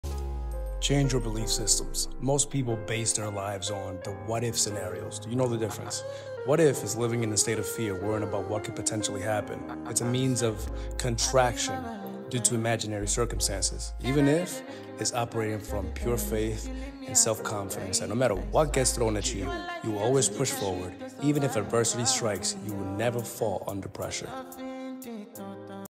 TRY THIS, MOVING FORWARD‼️✅mindset
CHANGE YOUR BELIEF SYSTEM‼️

Go from WHAT IF to EVEN IF.

One limits you and the other encourages you to push yourself past any limitations.